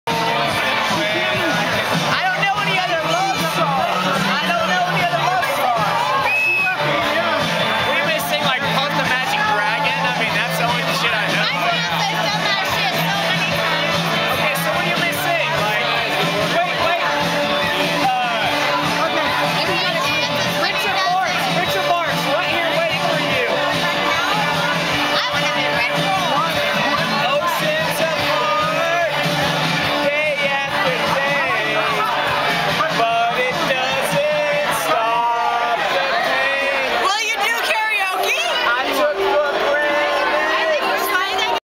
Nick and Damian singing to the Bachelorette (Randi) - Top Gun style- at the St. Patrick's Day Block Party in Dallas